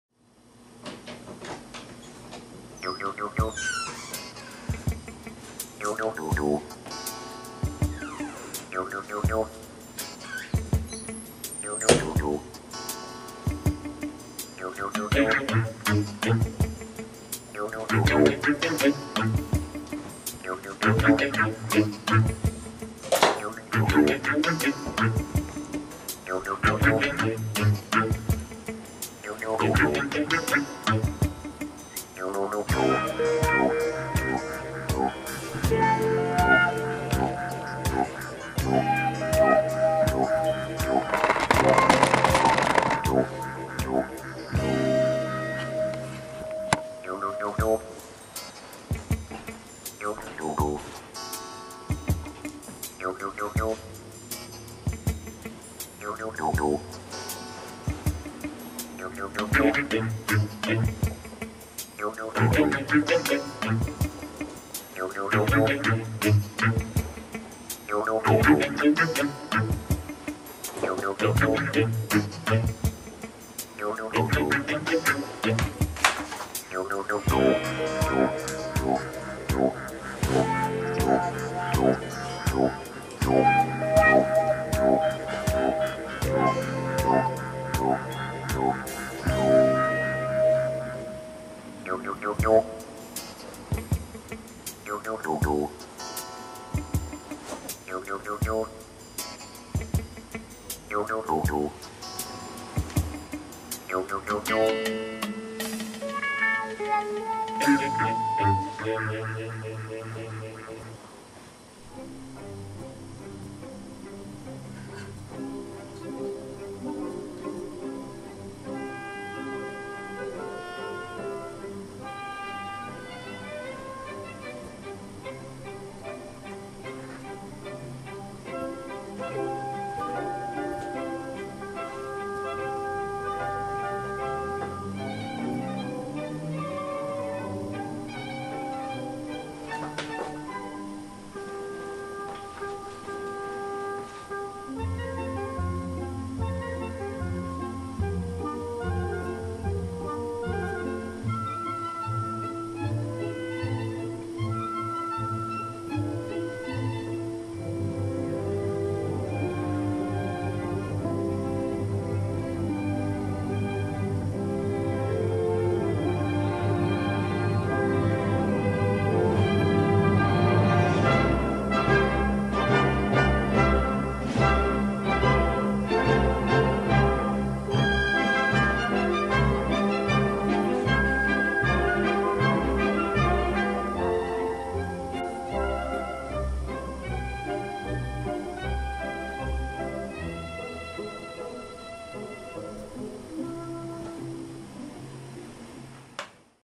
Zeke Wolf comes home from work
You may know Disney's Big Bad Wolf as a low-life ruffian sort of villain that is greedy and won't let anything get in the way of eating those pigs. But off-camera, he can be quite different...